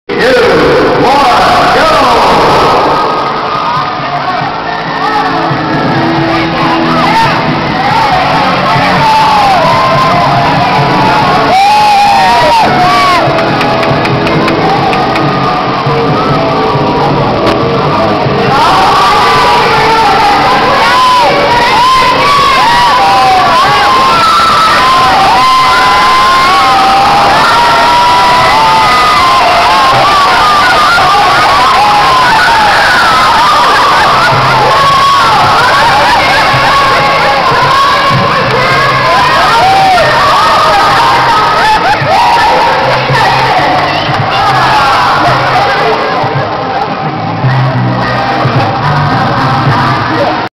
Ice Bed Races
Bed races at the Wichita Falls Wildcats game!